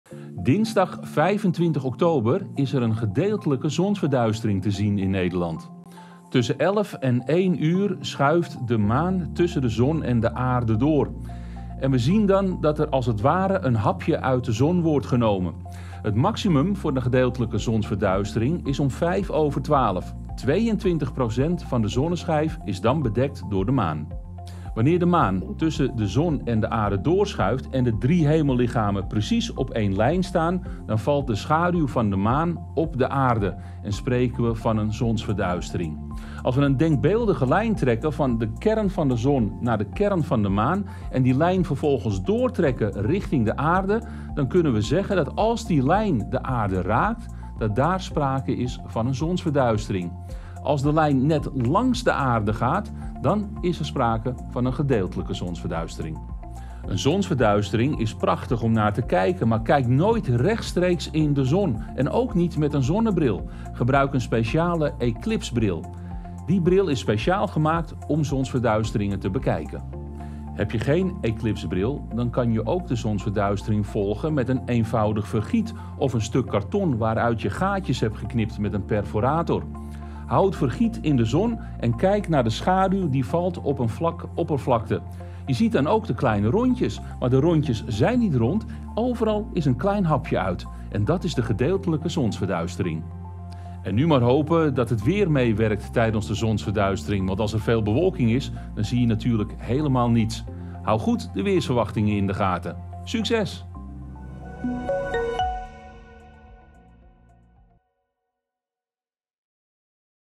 Gedeeltelijke zonsverduistering op komst! | Dinsdag 25 oktober
Volgende week dinsdag staat een gedeeltelijke zonsverduistering op het programma. Raymond Klaassen vertelt je er meer over in deze video!